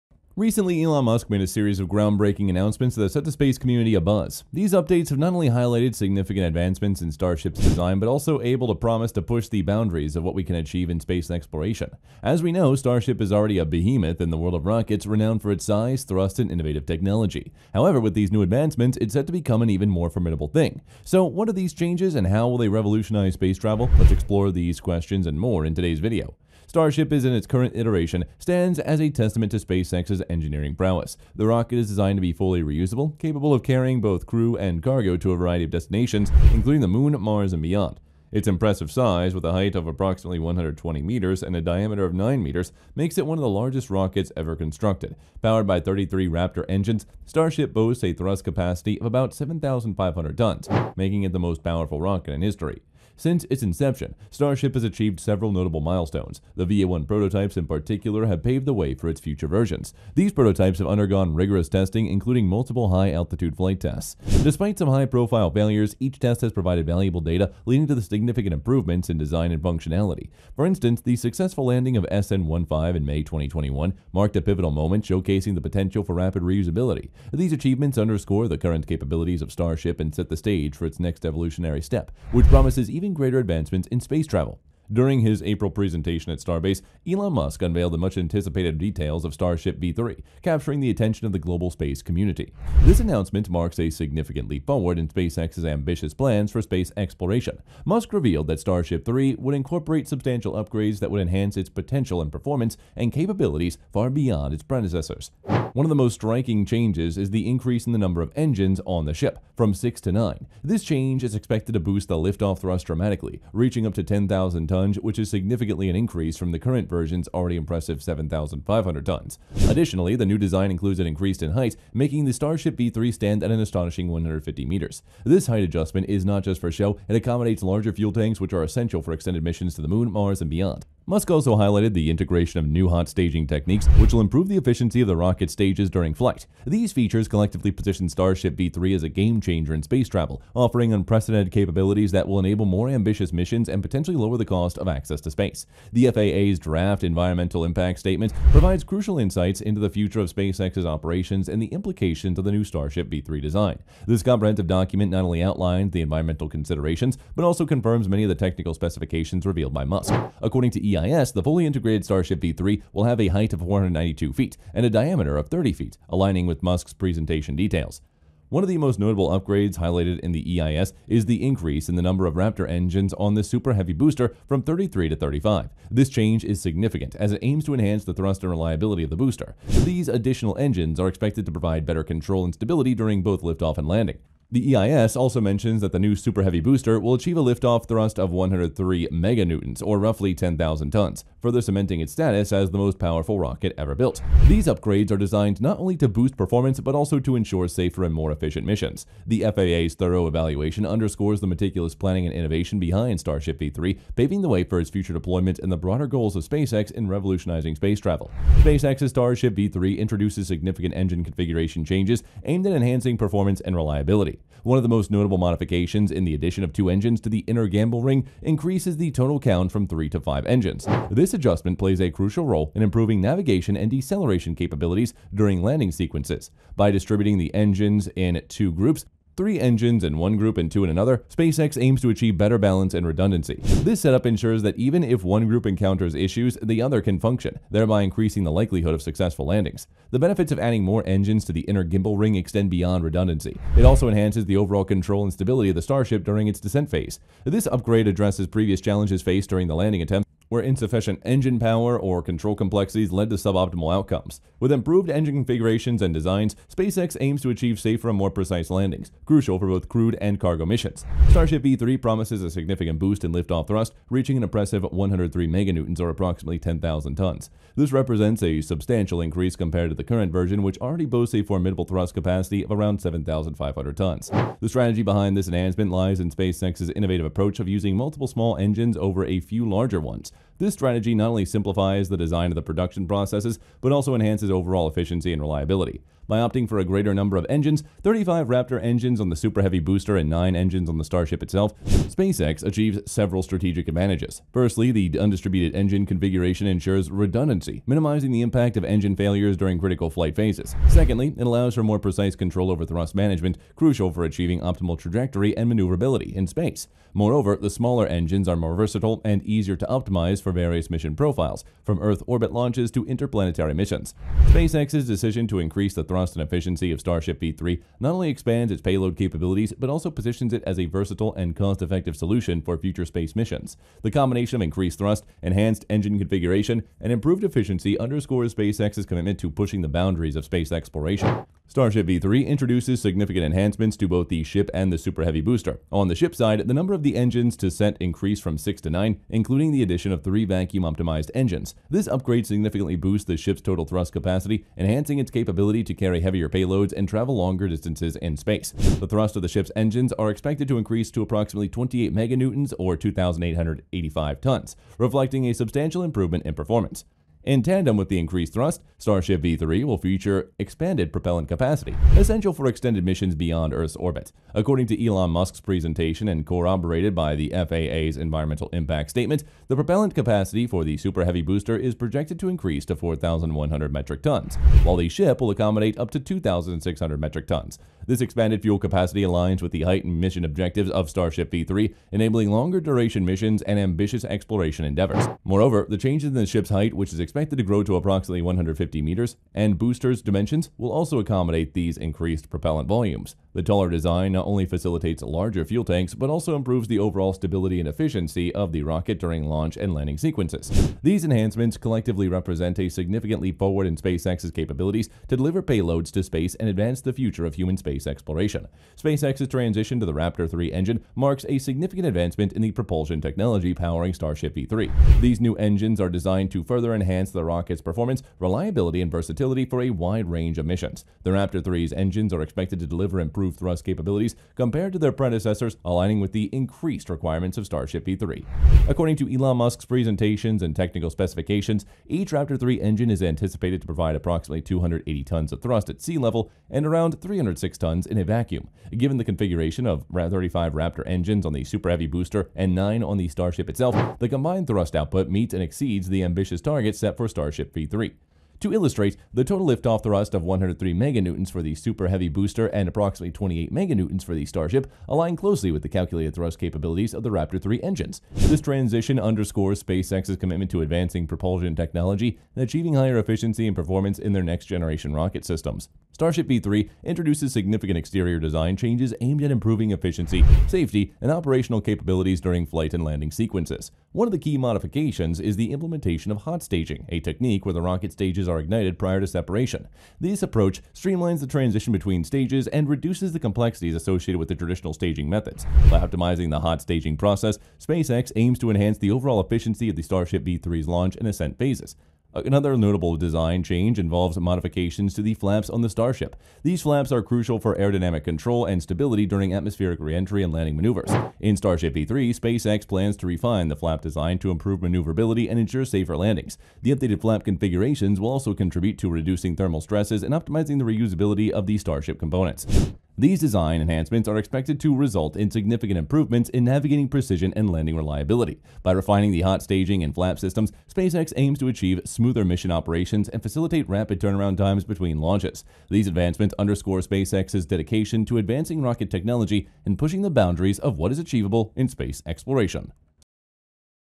Elon Musk Unveils Major Upgrades for Next Gen Starship Super Heavy
Recently, Elon Musk made a series of groundbreaking announcements that have set the space community abuzz. These updates not only highlight significant advancements in Starship’s design but also promise to push the boundaries of what we can achieve in space exploration. As we know, Starship is already a behemoth in the world of rockets, renowned for its size, thrust, and innovative technology. However, with these new enhancements, it's set to become even more formidable. So, what are these changes, and how will they revolutionize space travel? Let’s explore these questions and more in today’s video.